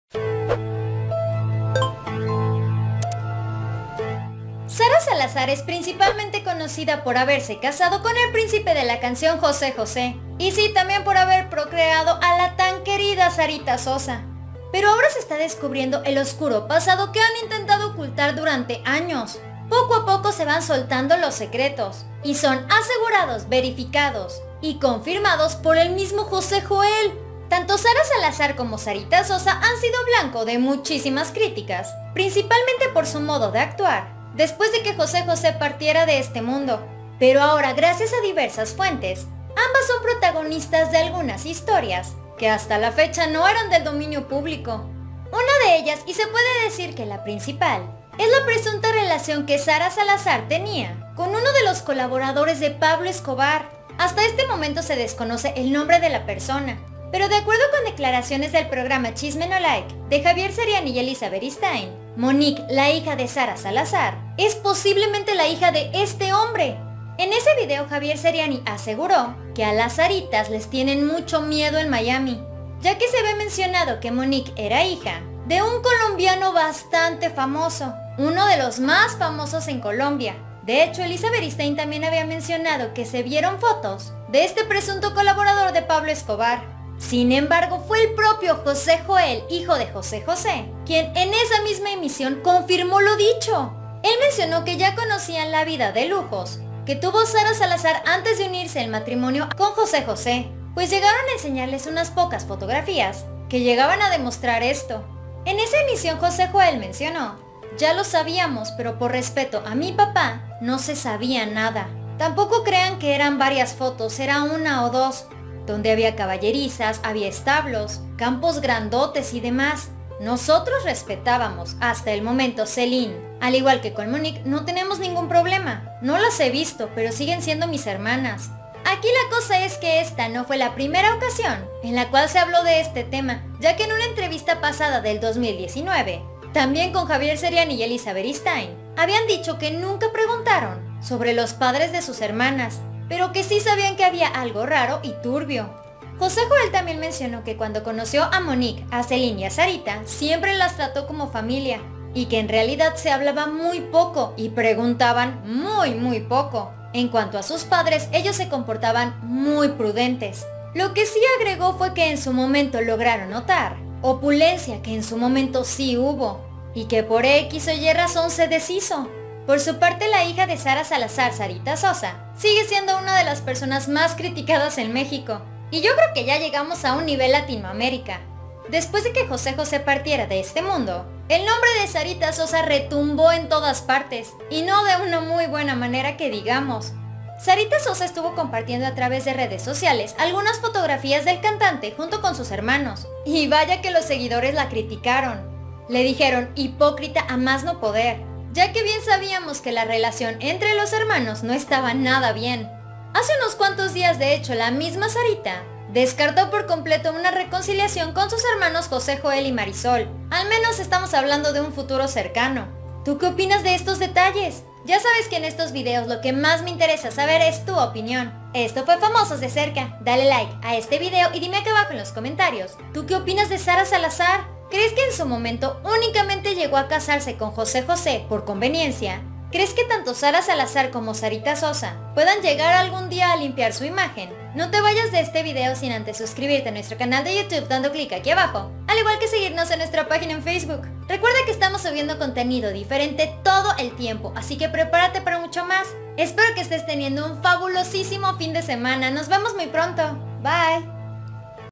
🔇🤐 ¡ 𝗦𝗘 𝗥𝗘𝗩𝗘𝗟𝗔 𝗧𝗢𝗗𝗔 𝗟𝗔 𝗩𝗘𝗥𝗗𝗔𝗗 🔕 Detrás De La Vida de 𝗦𝗮𝗿𝗮 𝗦𝗮𝗹𝗮𝘇𝗮𝗿 𝗩𝗶𝘂𝗱𝗮 𝗱𝗲 𝗝𝗼𝘀é 𝗝𝗼𝘀é ! 🤐🤐
Fuertes revelaciones de la viuda de José José salen a relucir hoy.

 BIENVENIDOS!!!